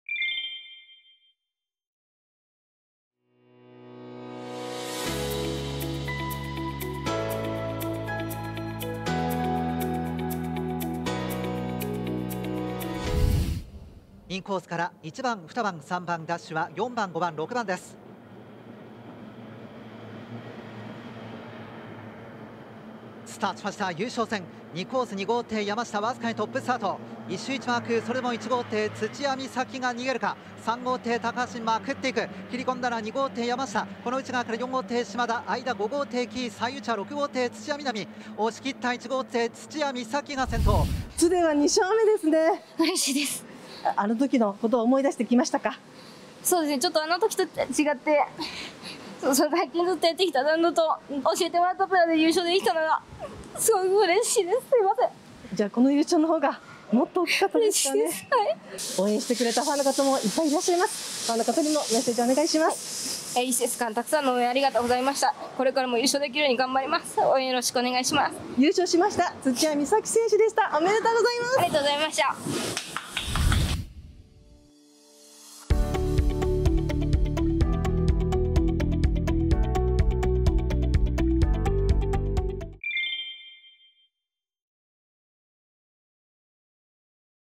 BOATCAST NEWS│手に汗握る 優勝戦！制したのは誰だ！？ ボートレースニュース 2021年11月22日│
優勝戦を制したのは誰だ！？
津 ヴィーナスシリーズ 最終日12Rをご覧ください。

最新のボートレースニュースをお届けします！